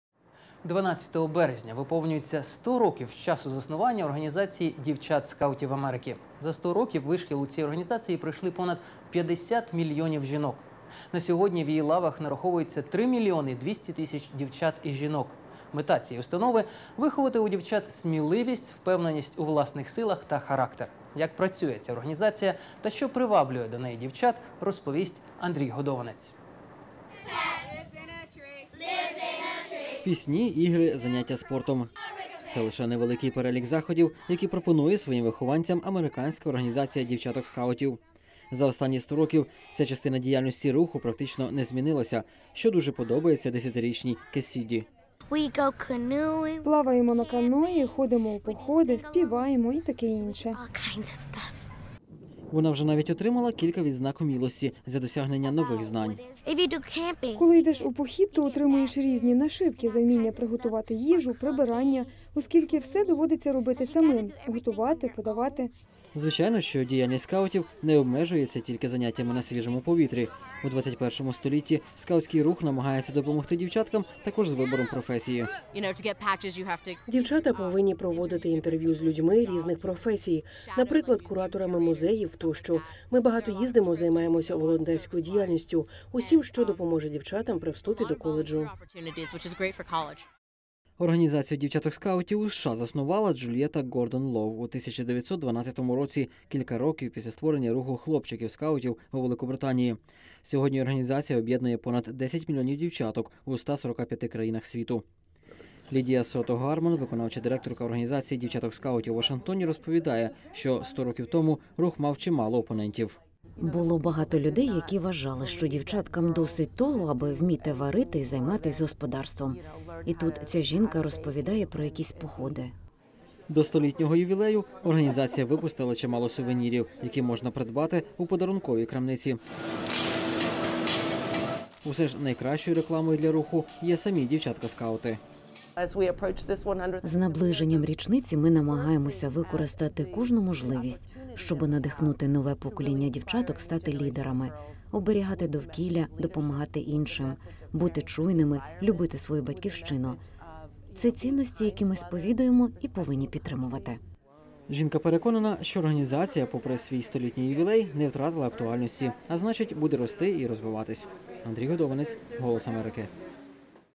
Cкаути у США відзначають 100-ліття
За 100 років вишкіл у скаутській організації пройшли понад 50 мільйонів  дівчат. На сьогодні в її лавах три мільйони 200 тисяч жінок.